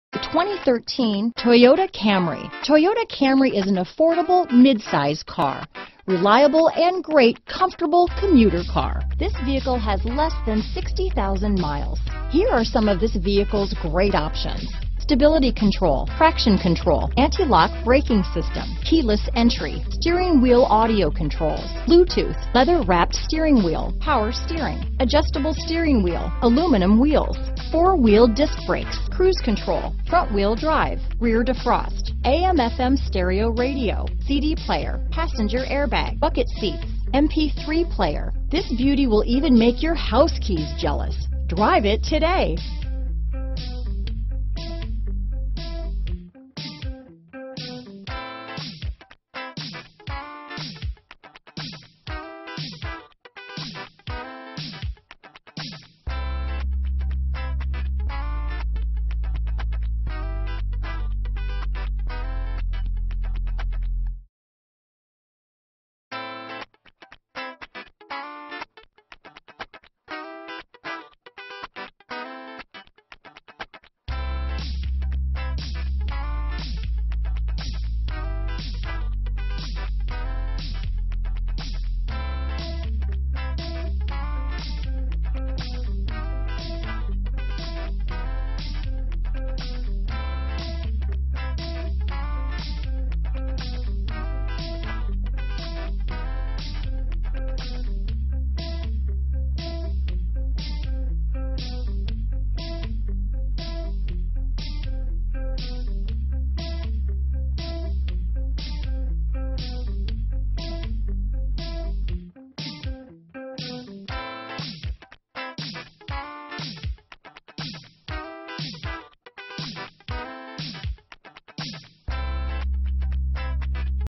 2013 Toyota Camry Peoria, Surprise, Avondale, Scottsdale, Phoenix, AZ P8204B
Solid and stately, this 2013 Toyota Camry banished all limitations in creating every last detail. With a Gas I4 2.5L/152 engine powering this Automatic transmission, it is an understated assertion of your dominion over the open highway. It comes equipped with these options: Whiplash-lessening front seats, Washer-linked variable intermittent windshield wipers, Vehicle stability control, Variable assist pwr rack & pinion steering, Unique SofTex sport fabric seat trim, Traction control, Tire pressure monitor system, Sport-tuned shocks & springs, Sport mesh front grille, and Smart stop technology . Visit Moore Chrysler Jeep at 8600 West Bell Road, Peoria, AZ 85382 today.